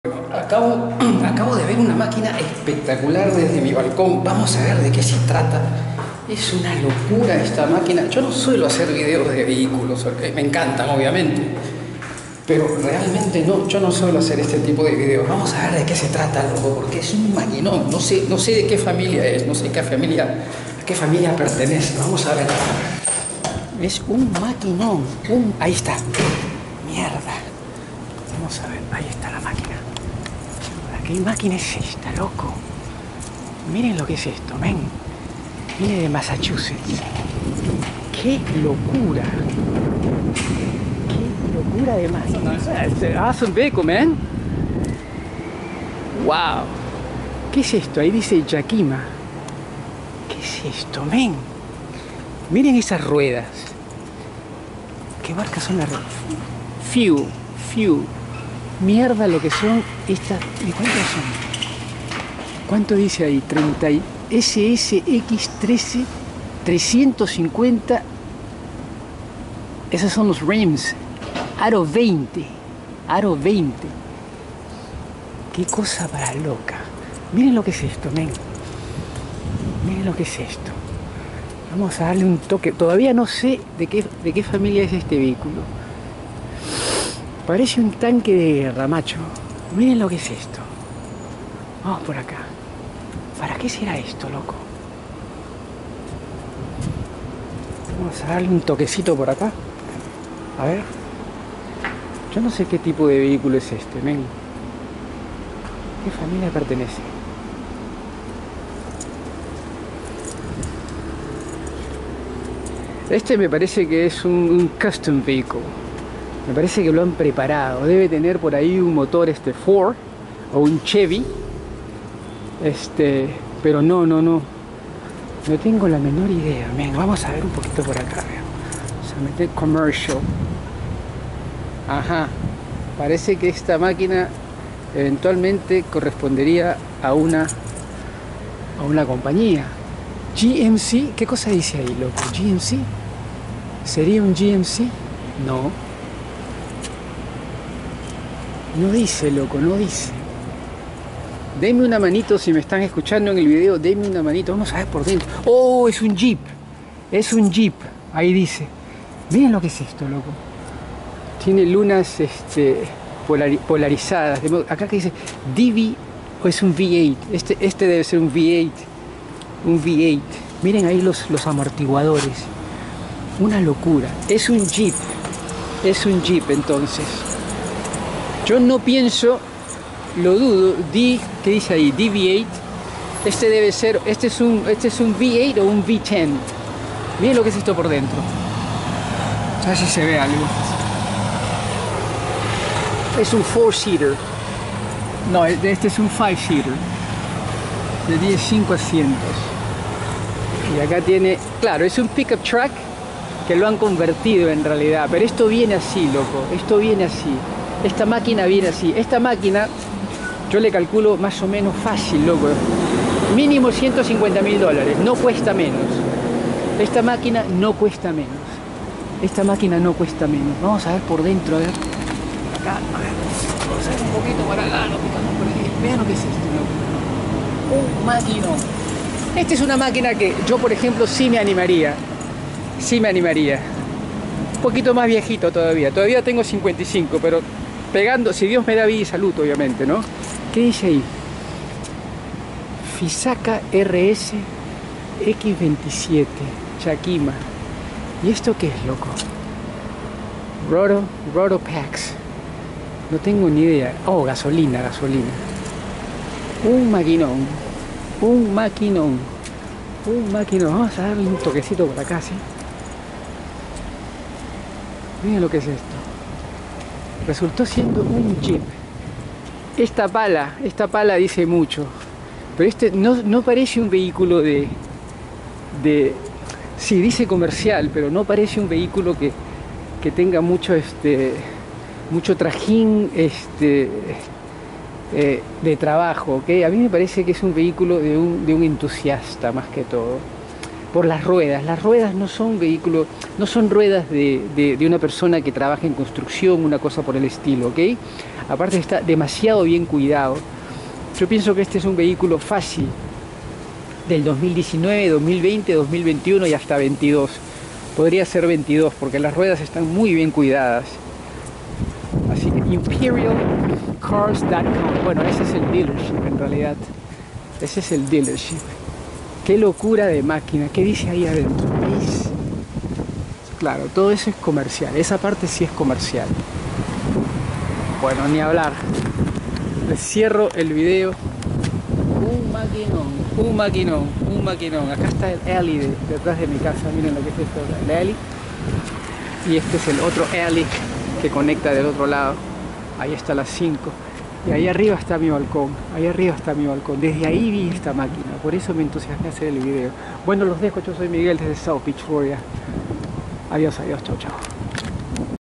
Me encontré con un MONSTRUO - Jeep Gladiator DV8 Offroad Customized - Un animal!
Jeep Gladiator DV8 Offroad Customized

Lo ví desde mi balcón y me mató! bajé a filmarlo, salí a hacer unas compras y al regresar, ya se había retirado; bingo! lo grabé justo a tiempo.